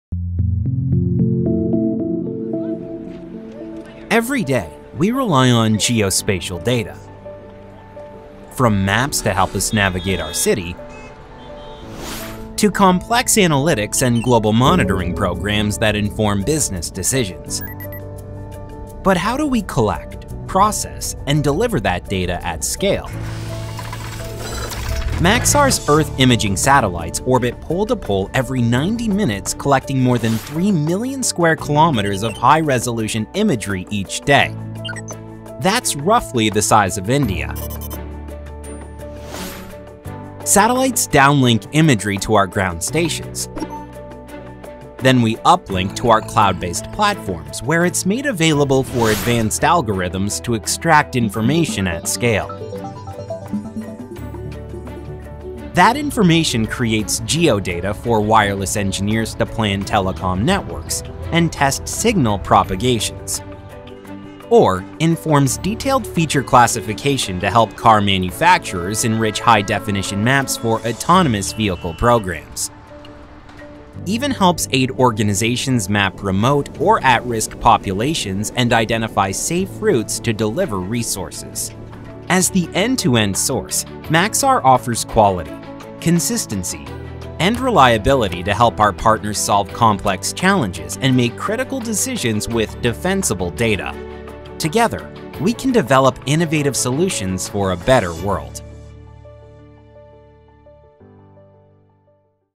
We are the Source - Telco
Every day, we rely on geospatial data. From maps to help us navigate our city, to complex analytics and global monitoring programs that inform business decisions. But how do we collect, process, and deliver that data at scale? Maxar's Earth imaging satellites orbit pole-to-pole every 90 minutes
collecting more than three million square kilometers of high-resolution imagery each day. As the end-to-end source, Maxar offers quality, consistency, and reliability to help our partners solve complex challenges and make critical decisions with defensible data. Together, we can develop innovative solutions for a better world.